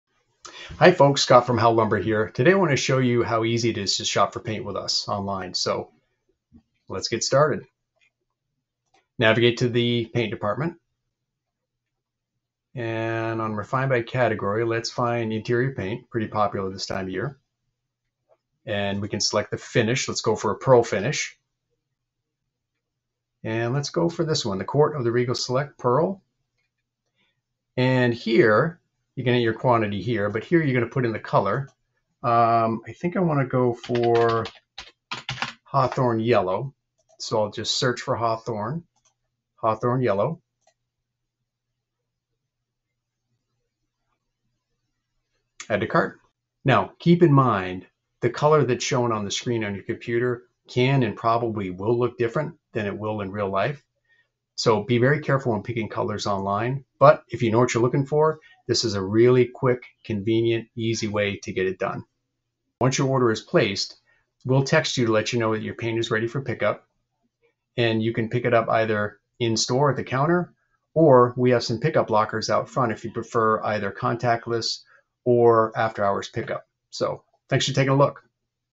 Buying Paint Online @ Howe Lumber - Wicked Easy!
A quick tutorial on paint shopping with us.

Howe Lumber is a Full Service Lumber Yard, Hardware Store & Building Supplier located in East Brookfield, MA.